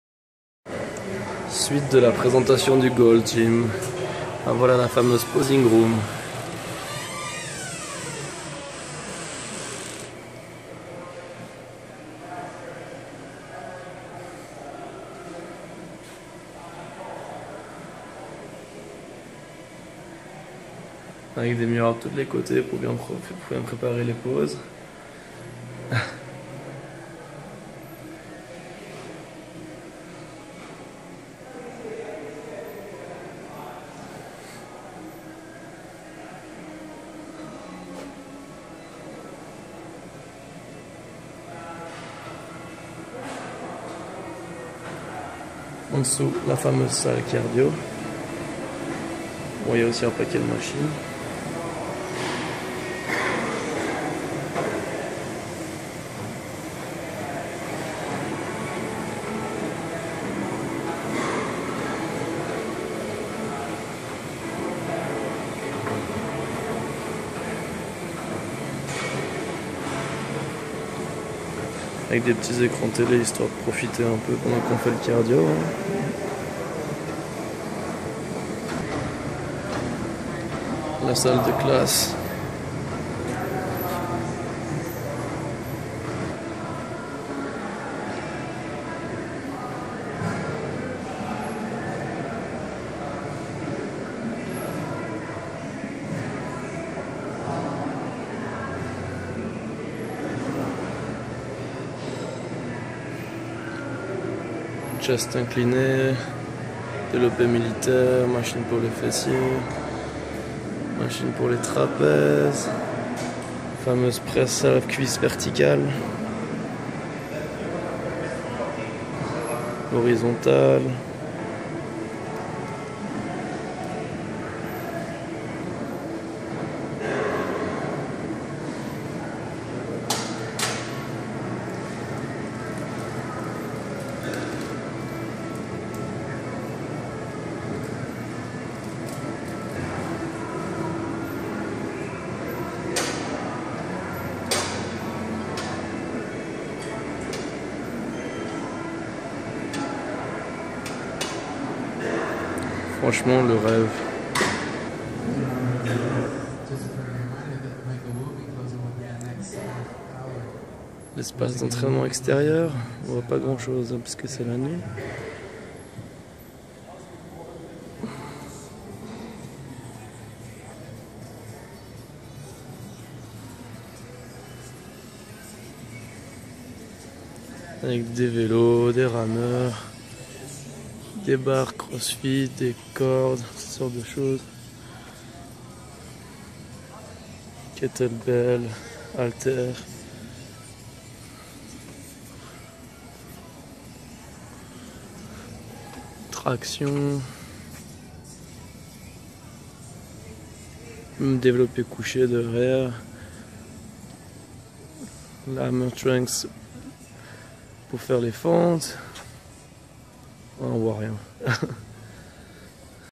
The Mecca of Bodybuilding : Gold's gym Venice Pt.3
Presentation of the 3rd room, posing room and outside space